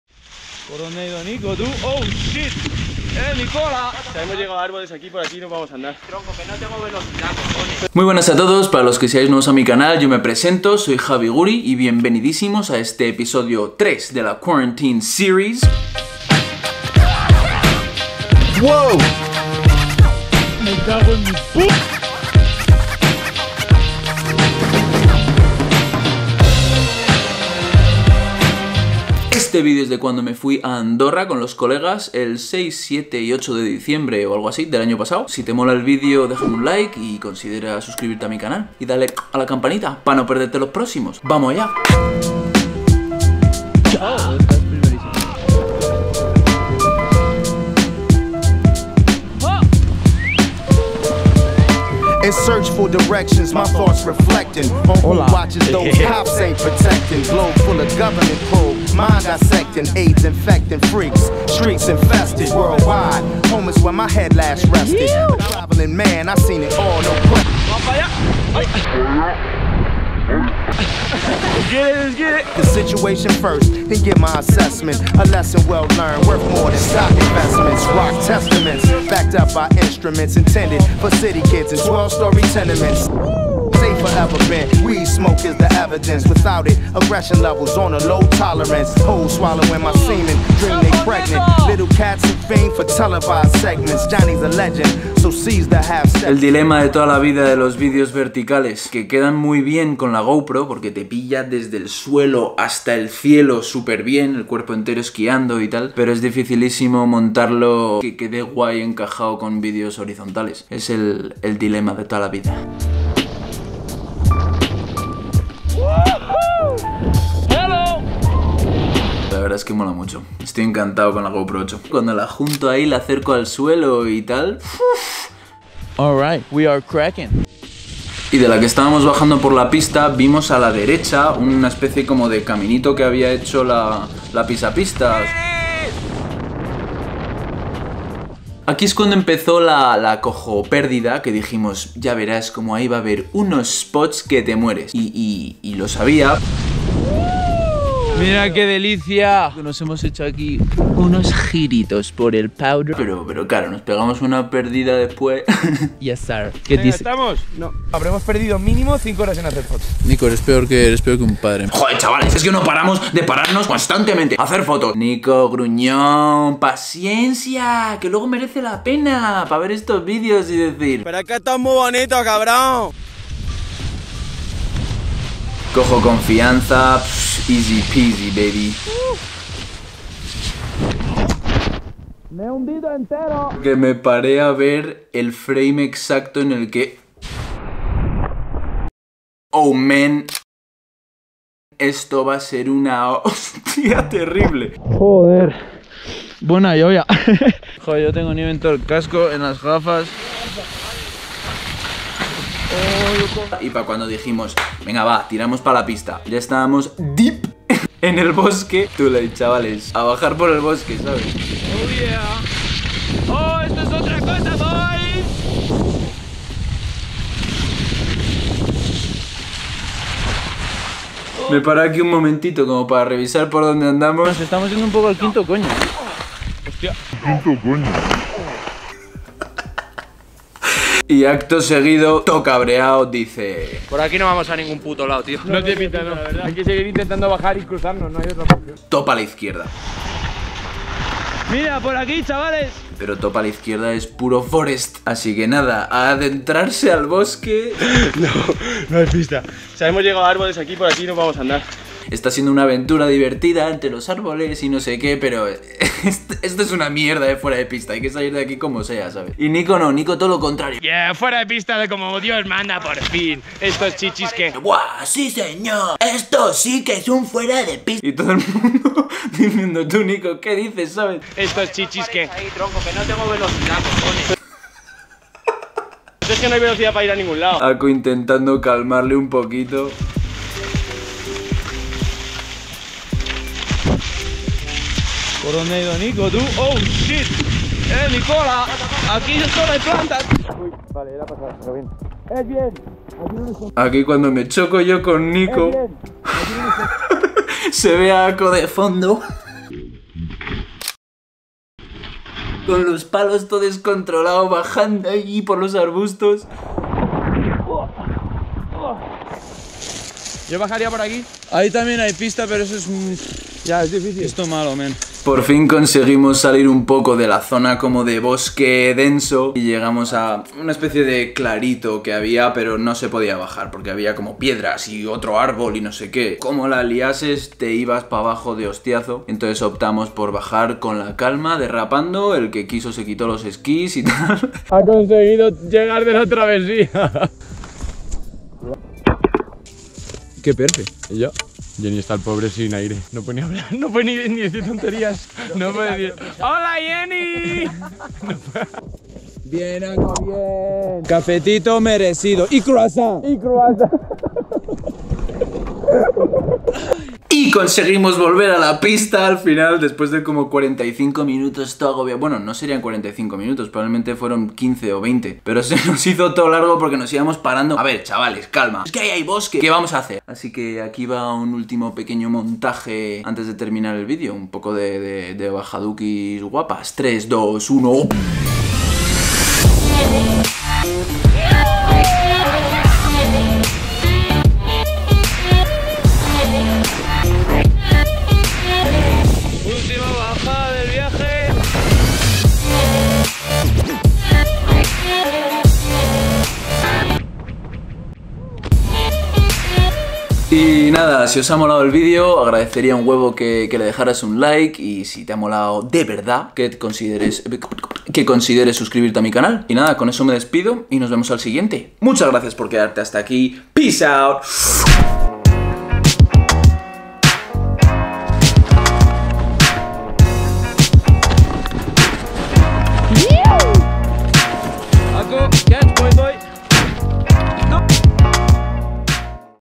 Nos PERDIMOS EN UN BOSQUE ESQUIANDO | Quarantine Series Ep. 3 - Andorra
Buscando nieve intacta por fuera de pista acabamos perdidos bajando por un bosque, y ha dado para un video bastante divertido jejeje.. Una pequeña dosis de esquí en estos tiempos de confinamiento además ¿tampoco viene del todo mal, no?
Espero que os mole el video!